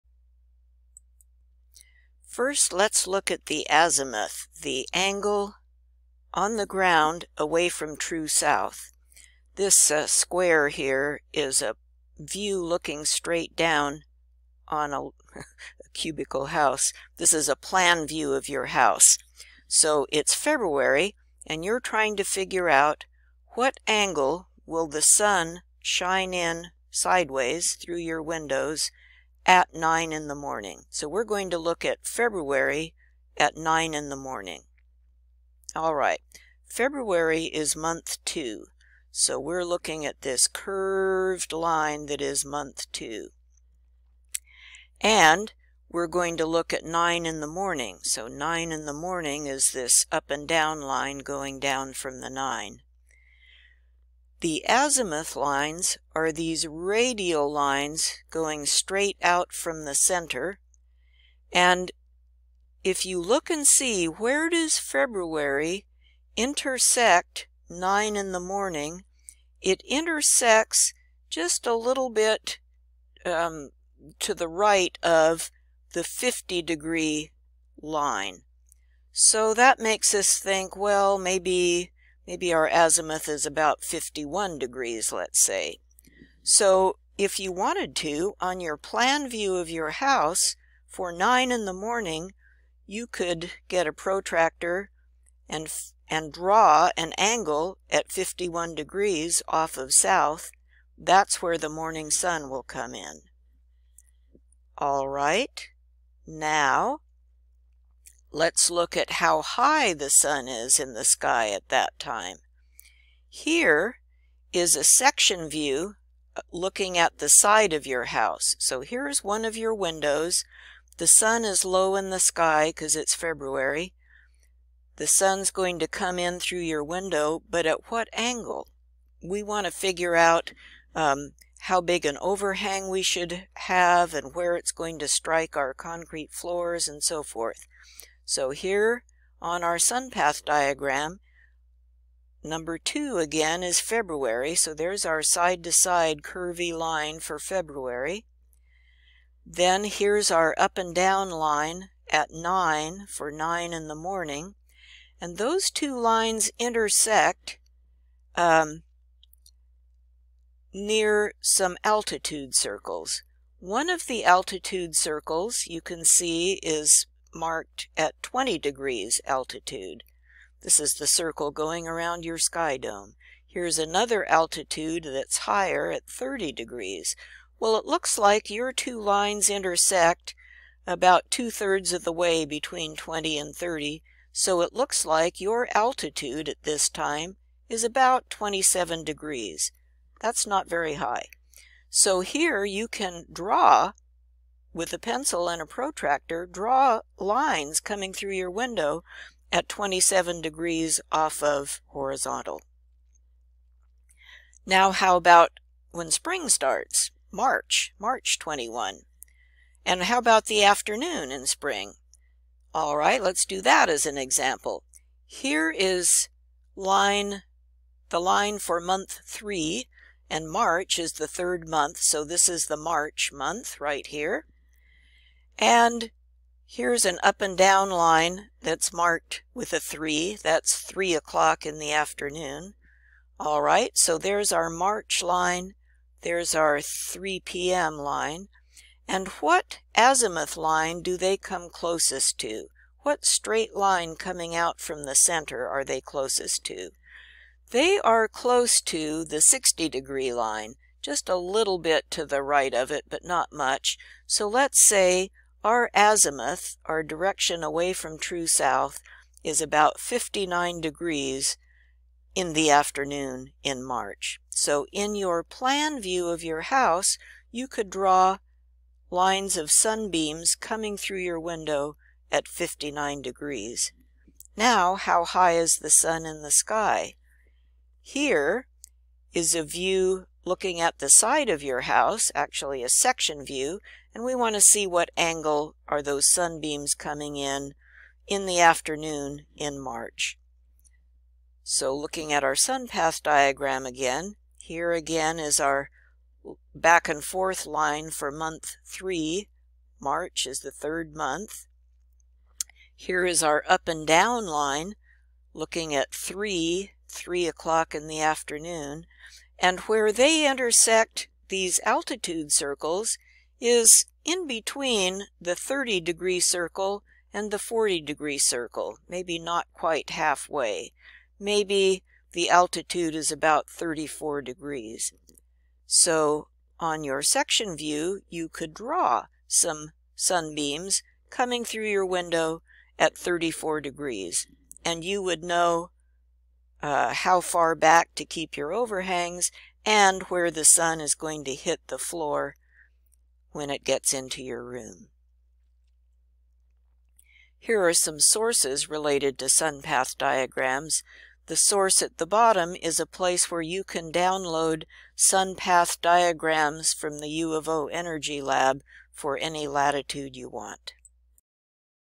SBS07a sun path diagrams 2 VL
Learn to use a sun path diagram as the first step in passive solar heating design.